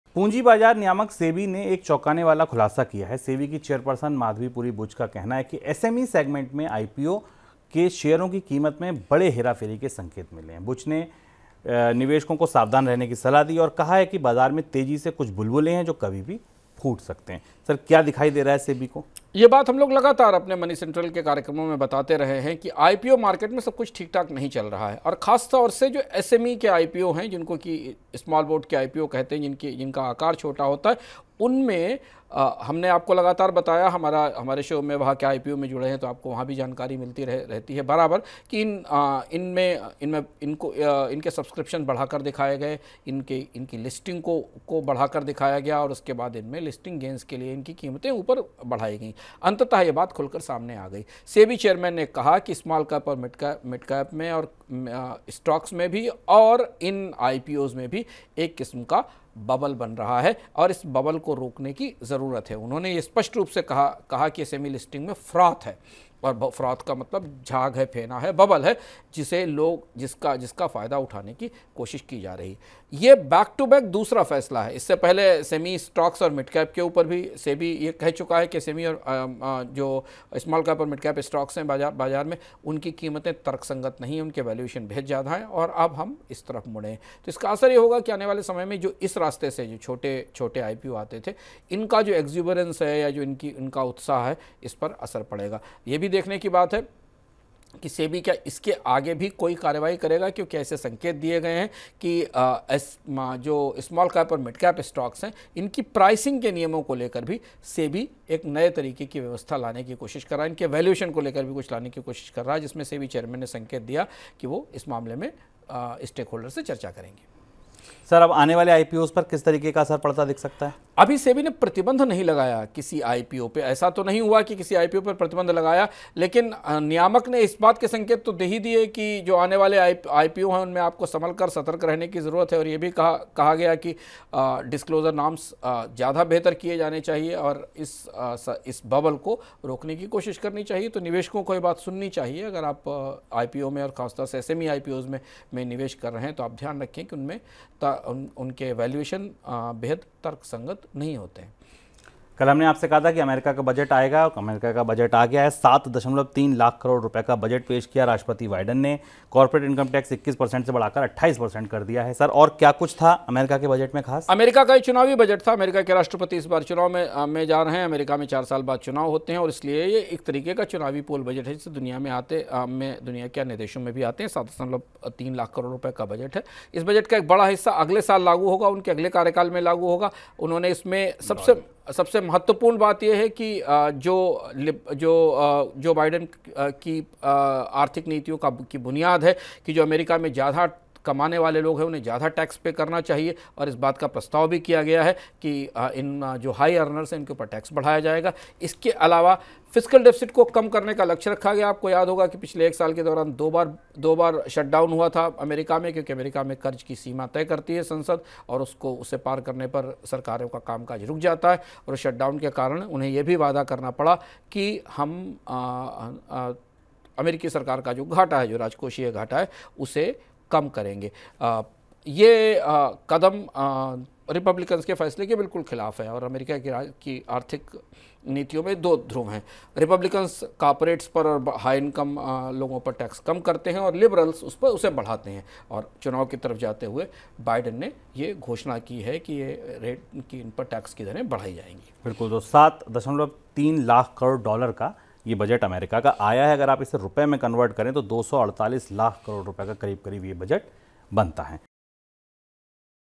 SME IPO में घोटाले को लेकर SEBI को क्या शक? Money9 Explained | Anshuman Tiwari
पूंजी बाजार नियामक सेबी ने एक चौंकाने वाला खुलासा किया है. सेबी की चेयरपर्सन माधवी पुरी बुच का कहना है कि SME सेगमेंट के IPO और शेयरों की कीमतों में हेराफेरी के संकेत मिले हैं. बुच ने निवेशकों को सावधान रहने की सलाह दी है और कहा है कि बाजार में तेजी के कुछ बुलबुले हैं जो कभी भी फूट सकते हैं.